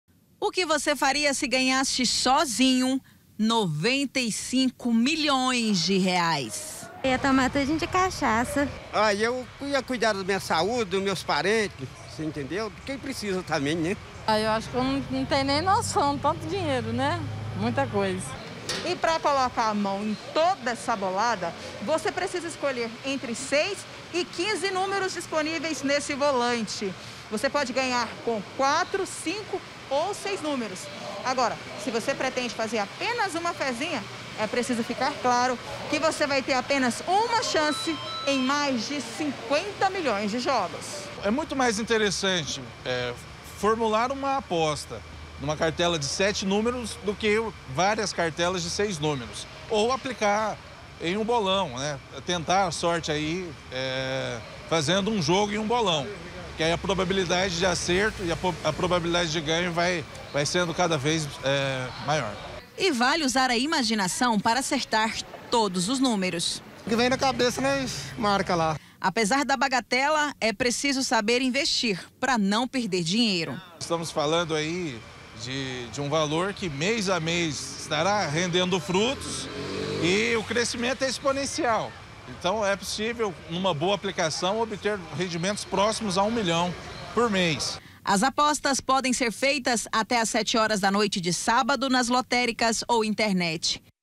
MEGA SENA PRÊMIO VAI SER DE 95 MILHÕES
E por falar em se reinventar, você já pensou receber um milhão de reais por mês para o resto da vida sem trabalhar? Pois é, quem ganhar na Mega Sena acumulada vai ter essa chance.  O prêmio é de 95 milhões de reais, mas é preciso uma boa estratégia no jogo e muita sorte. Se ganhar, tem que saber investir não é mesmo?